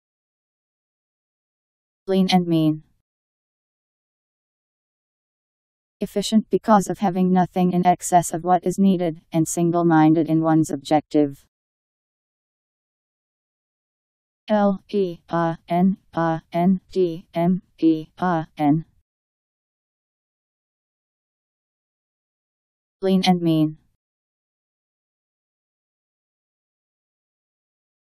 Lean and mean Meaning
Video shows what lean and mean means. efficient because of having nothing in excess of what is needed, and single-minded in one's objective.  Lean and mean Meaning. How to pronounce, definition audio dictionary. How to say lean and mean. Powered by MaryTTS, Wiktionary